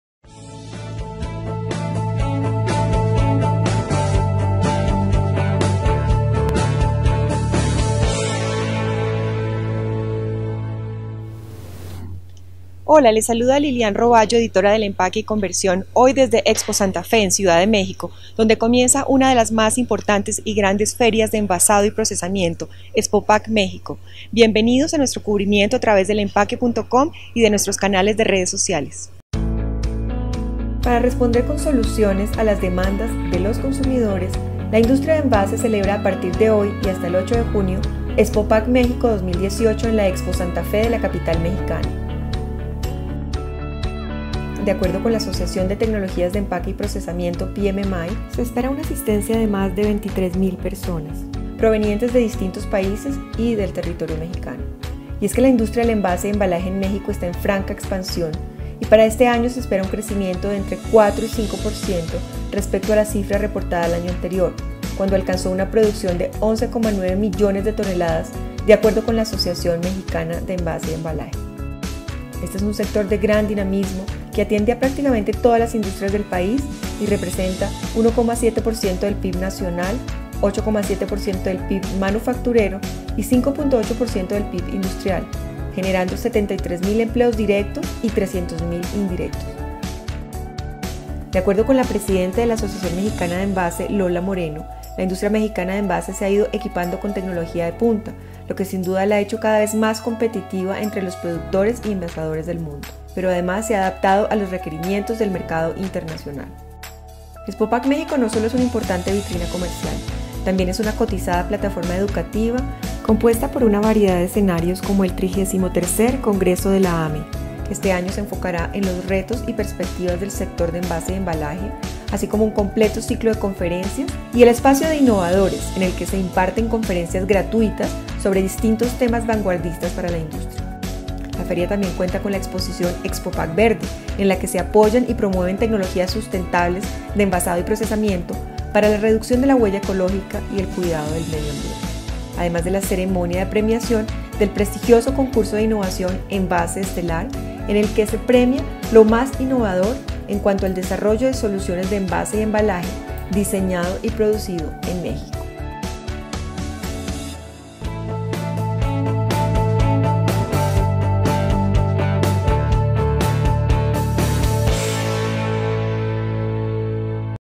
¡Inicia Expo Pack México 2018!
El Empaque+Conversión está presente en Expo Pack México 2018 para realizar un completo cubrimiento de esta importante feria para las industrias del envasado y procesamiento. ¡No se lo pierda!